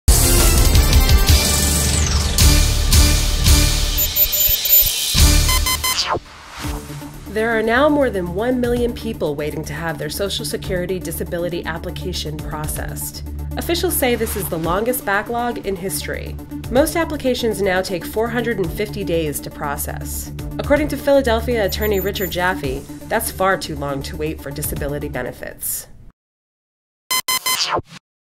Historic Social Security Disability Backlog | Law Wire News | November 2015
The backlog to process applications for Social Security Disability benefits exceeds more than one million people, the largest backlog in history, according to officials. Most applications now take 450 days to process. That’s far too long to wait, according to Philadelphia Social Security Disability lawyer Richard Jaffe.